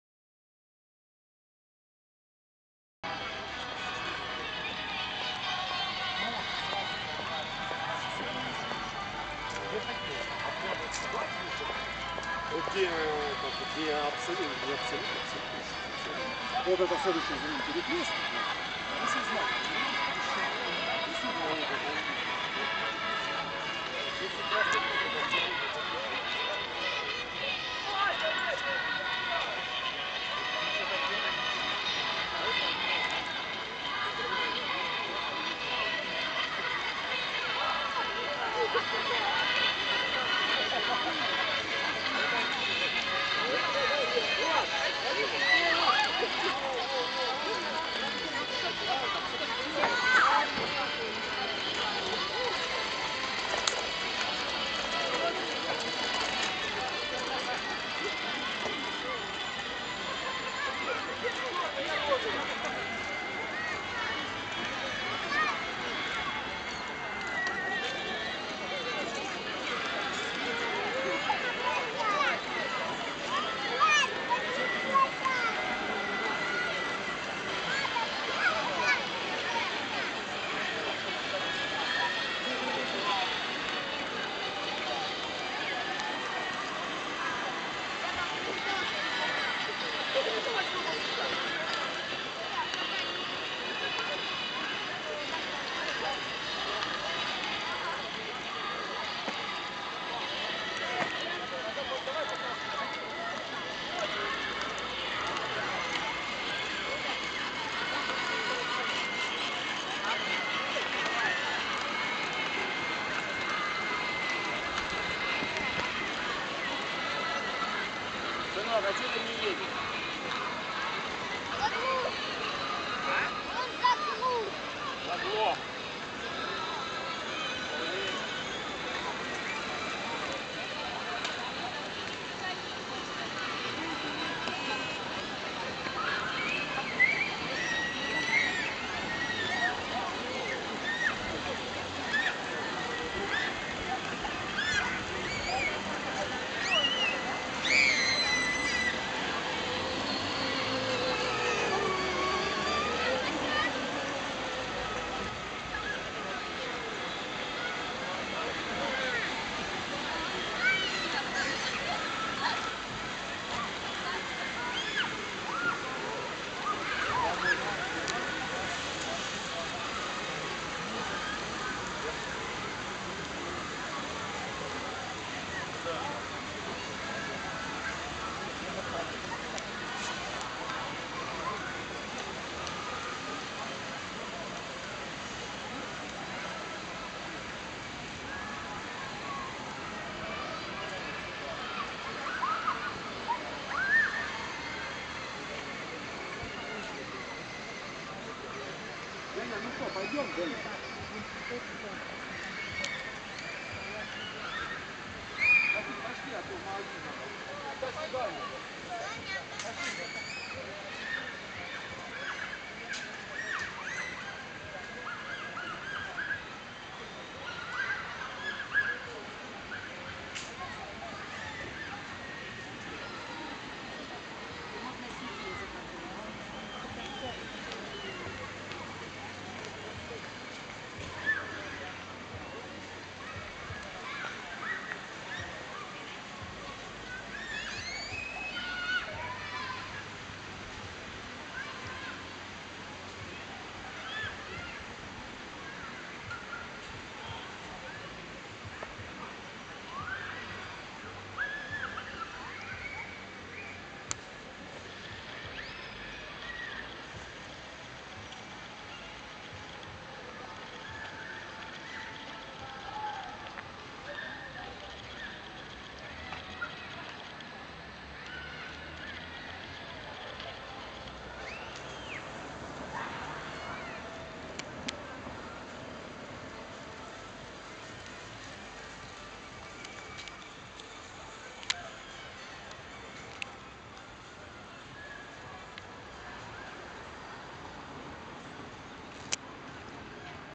Aug 22nd Slavyansk, Ukraine: Central square/Славянск, центральная площадь
22nd August 2014 Central  Square in Slavyansk, East  Ukraine.
City is not so peaceful as it may look. Ukrainian Army occupied Slavyansk after People's Militia left  the  city on July 5th.
Ukrainian flags all over  the city & painting yellow-blue  "national colors" don't make people accept Kiev's government.
Residents  of Slavyansk are waiting impatiently for People's Militia to return and reinstall Donetsk Repubic, independent from Kiev & rest of Ukraine.

During seige of Slavyansk, church on the central square was used for funeral services for the city residents killed  by Ukrainian artillery shelling.

22 августа 2014 Славянск, центральная  площадь
Обилие  украинской символики вовсе не означает, что Славянск сдался. Жители  с нетерпением ждут возвращения ополчения.

Во время осады Славянска в церкви  на площади проходили  поминальные  службы по убитым во время обстрелов города  украинской артиллерией мирным жителям  и ополченцам.